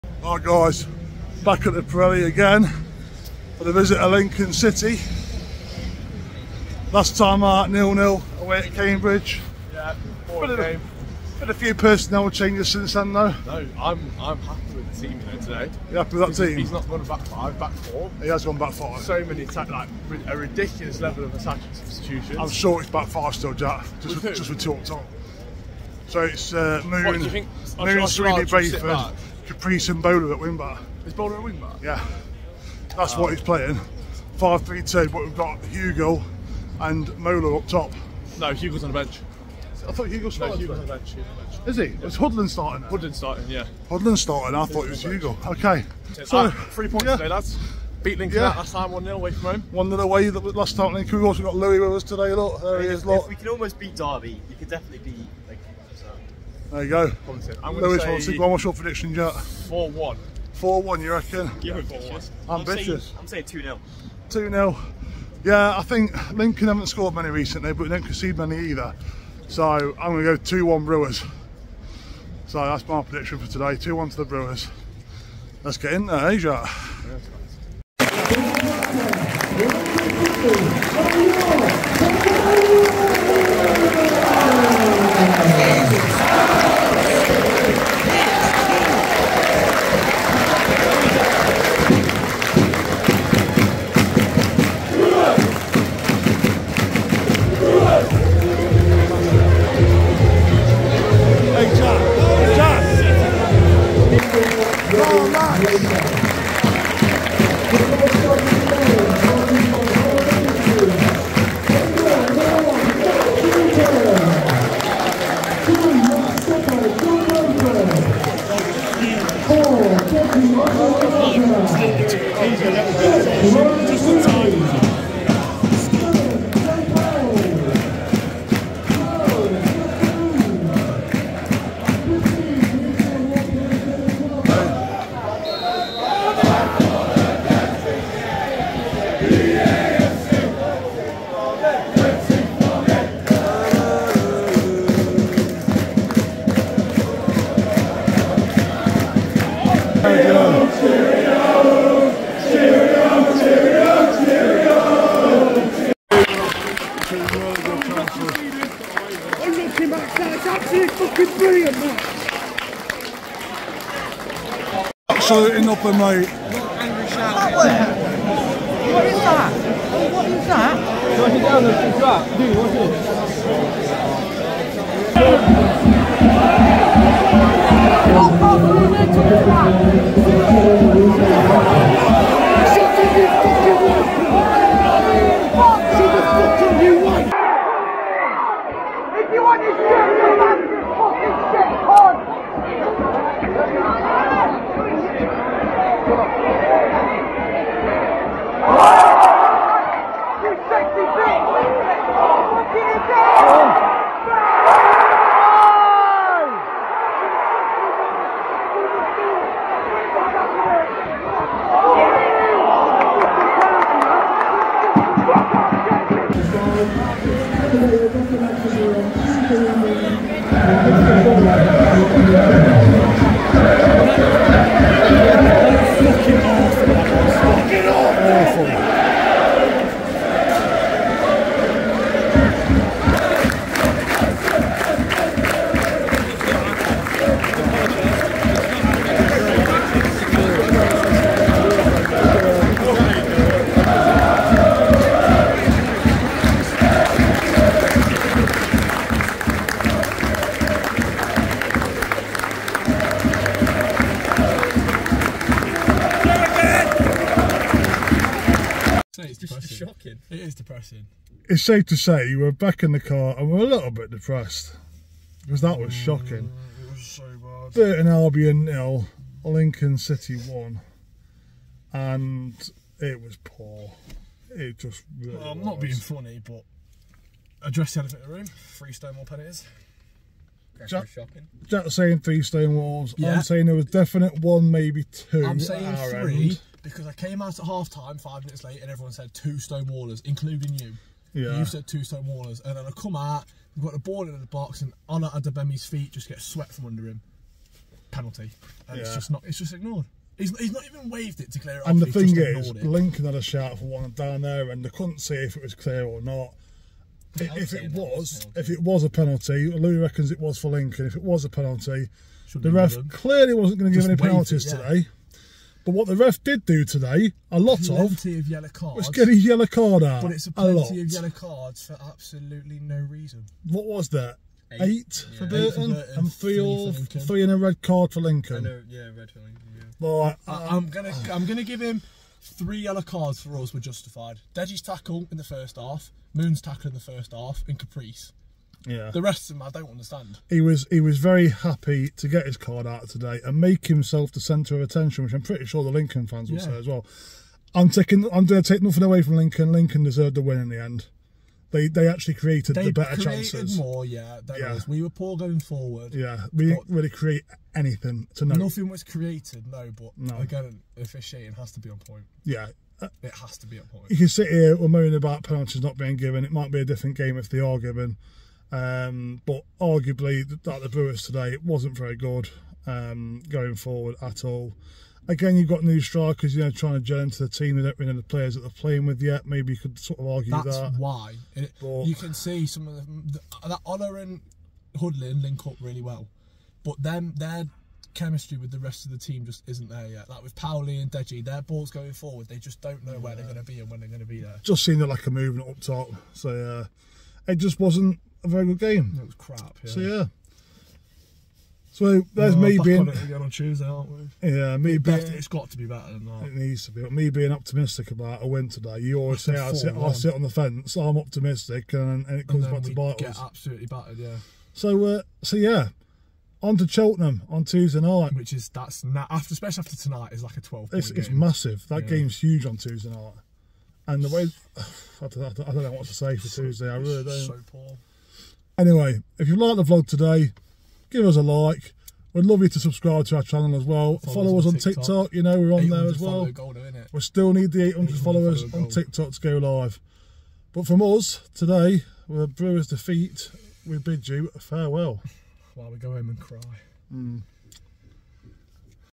Defeat for the Brewers | Burton Albion vs Lincoln City VLOG!
Thanks for checking out todays vlog as the Brewers play Lincoln!